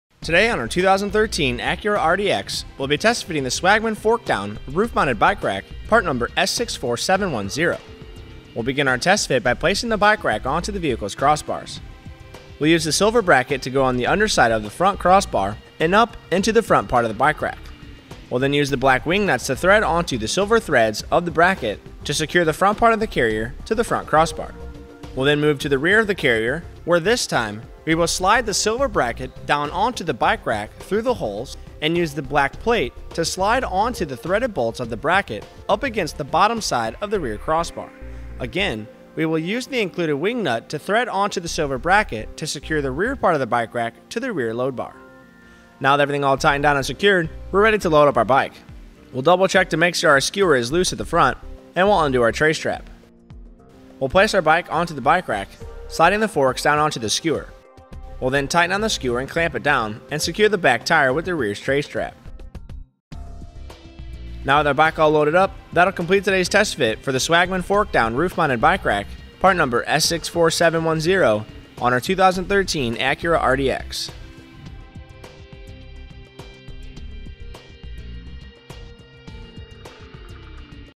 etrailer | Is the Swagman Fork Down Roof Mounted Bike Carrier Compatible with the 2013 Acura RDX?
Today on our 2013 Acura RDX we'll be test fitting the Swagman fork-down roof-mounted bike rack, part number S64710. We'll begin our test fit by placing the bike rack onto the vehicles crossbars.  We'll use the silver bracket to go on the underside of the front crossbar and up into the front part of the bike rack.  We'll then use the black wing nuts to thread onto the silver threads of the bracket to secure the front part of the carrier to the front crossbar.  We'll then move to the rear of the carrier, where this time we will slide the sliver bracket down onto the bike rack through the hole and use the black plate to slide onto the threaded bolts of the bracket up against the bottom side of the rear crossbar. Again, we will use the included wing nut to thread onto the silver bracket to secure the rear part of the bike rack to the rear load bar.  Now with everything all tightened down and secured we're ready to load up our bike.  We'll double check to make sure our skewer is loose at the front and we'll undo our tray strap.

We'll place our bike onto the bike rack sliding the forks down onto the skewer.  We'll then tighten down the skewer and clamp it down and secure the back tire with the rear tray strap. Now with our bike all loaded up that will complete today's test fit for the Swagman fork-down roof-mounted bike rack, part number S64710 on our 2013 Acura RDX.